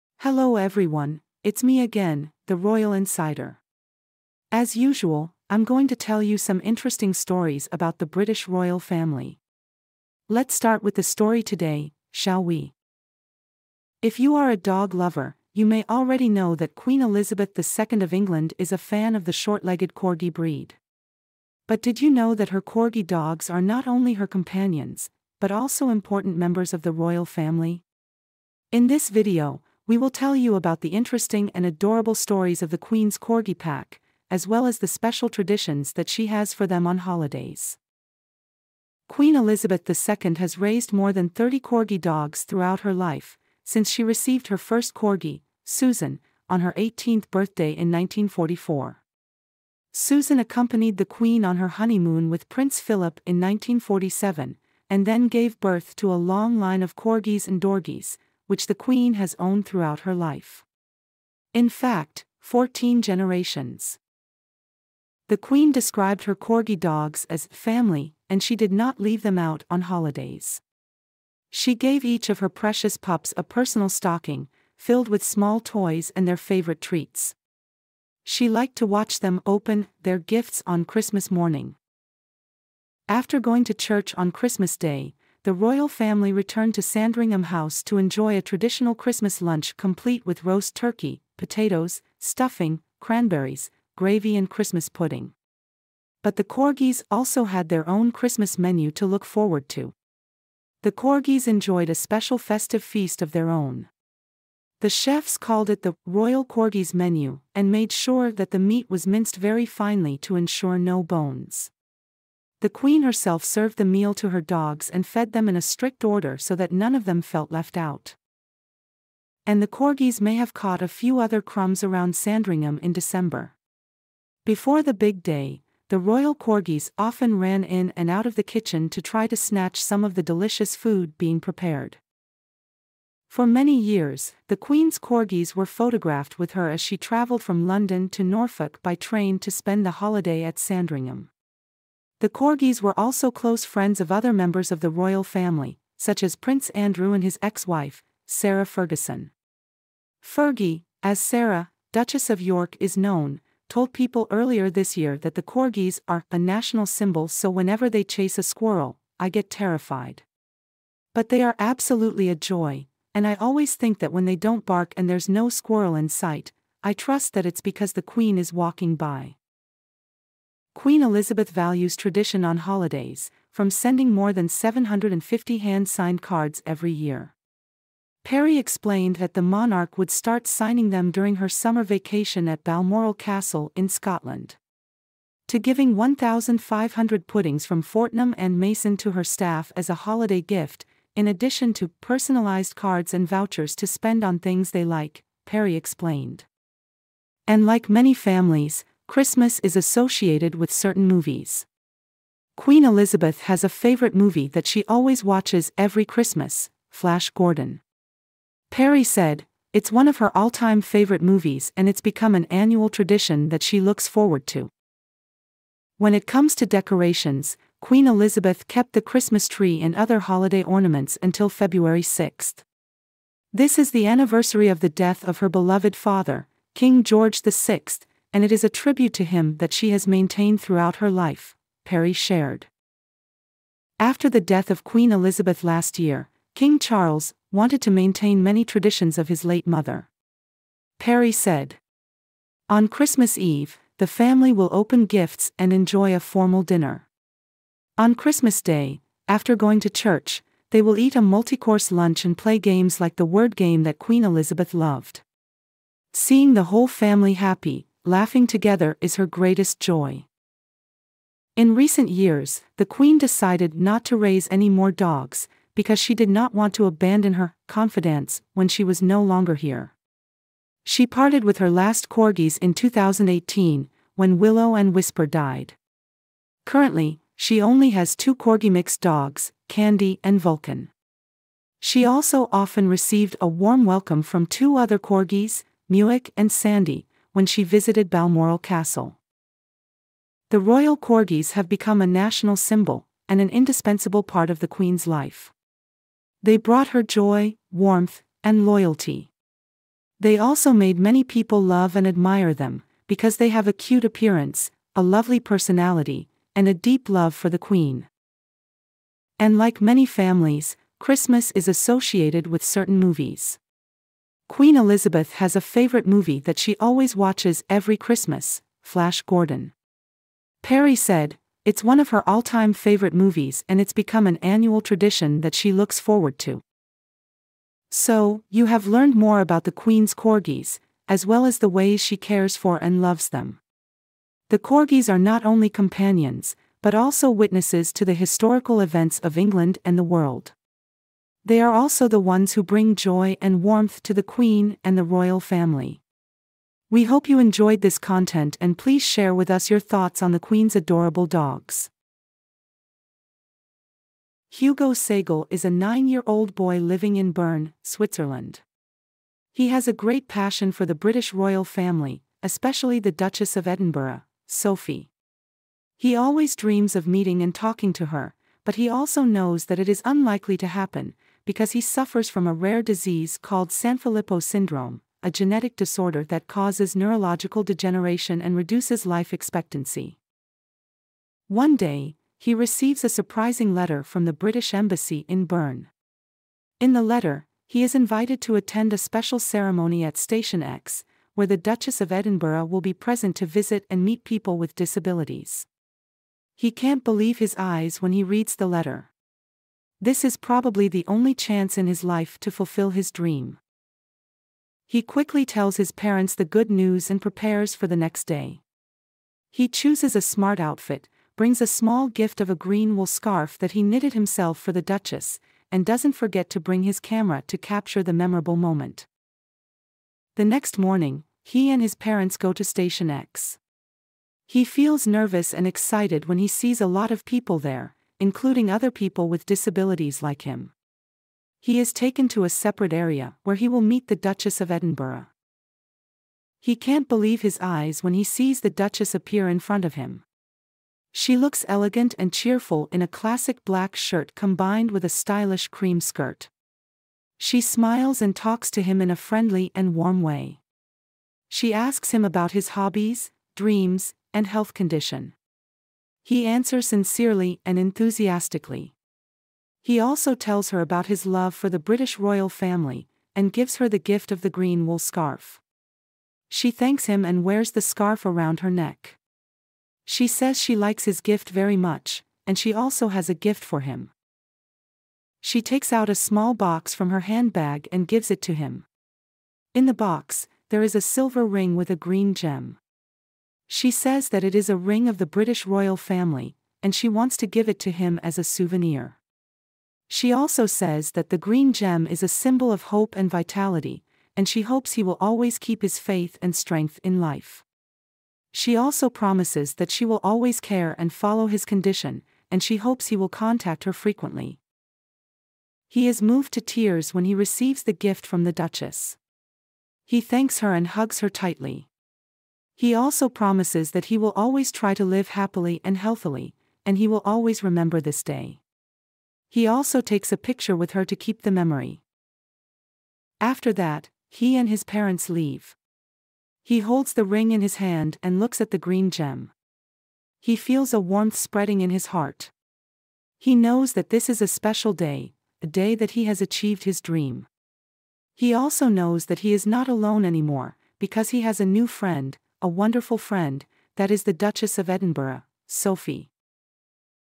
Queen Elizabeth gave 'Personalized Stocking' for Each Corgi at the Royal Christmas party
00:00 - Queen Elizabeth gave 'Personalized Stocking' for Each Corgi at the Royal Christmas party
6:00 - Duchess Sophie's special gift to young royal fan will bring tears to your eyes